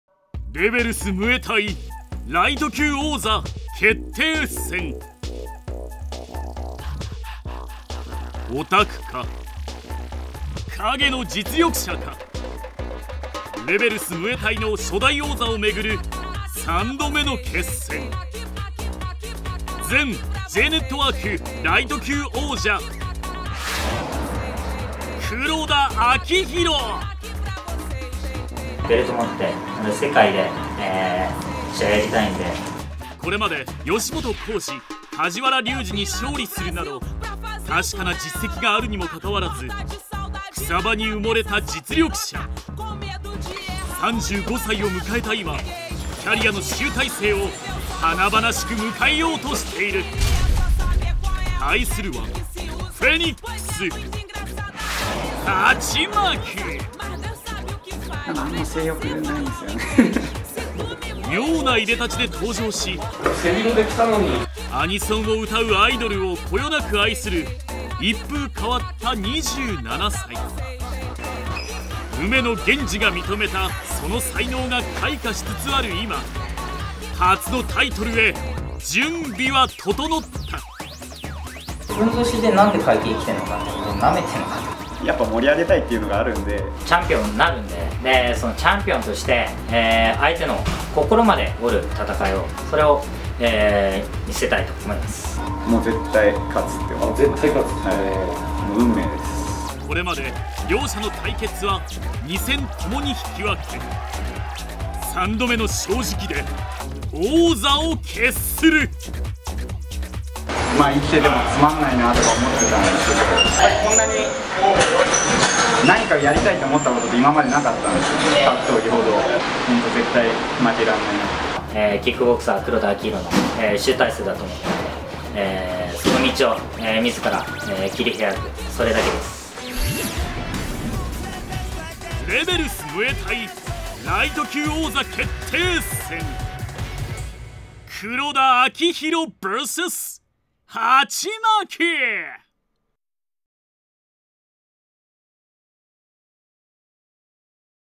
REBELS.17 ハチマキvs黒田アキヒロ トレイラー
2013年7月21日に東京・ディファ有明で行われるREBELS.17。
REBELS-MUAYTHAIライト級王座決定戦が昼の部のメインイベント。

３度目の対決は王座を争う大舞台！
"いけてない軍団 筆頭"ハチマキ vs
"元J-NETWORKライト級王者"黒田アキヒロ。